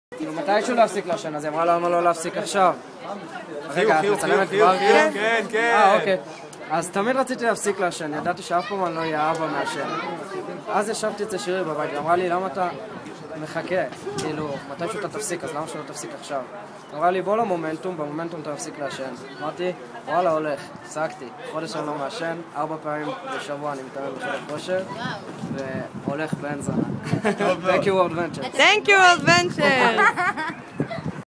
חברי וורלד ונצ׳רס מפסיקים לעשן!
כשאתה מסתובב עם אנשים שמקדמים אותך למטרות שלך, אתה פתאום מתחיל ליישם!!